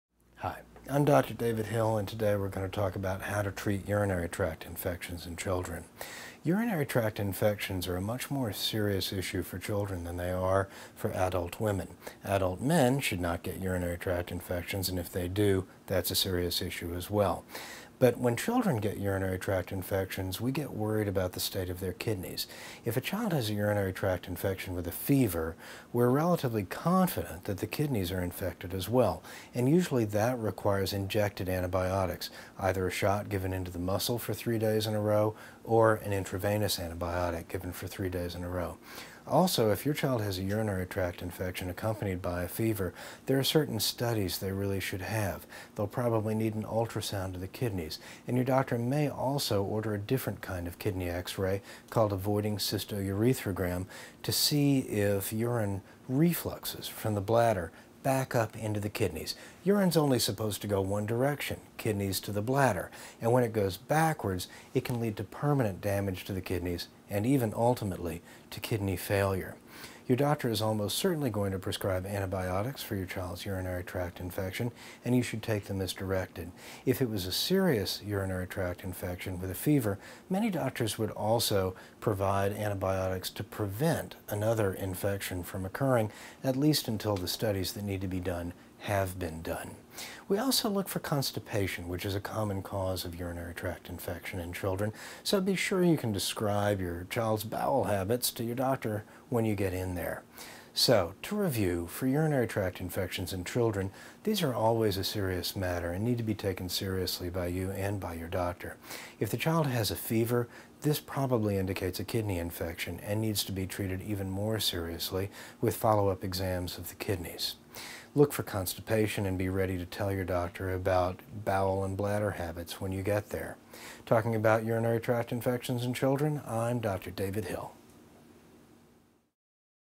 Kids' Health & Pediatrics : How to Treat Urinary Tract Infections in Kids
Urinary tract infections for children are a much more serious issue than they are for adults, and they may require a shot of antibiotics. Discover why a urinary tract infection in a child usually means that their kidneys are infected, as well, with help from a pediatrician in this free video on preventing medical problems in children.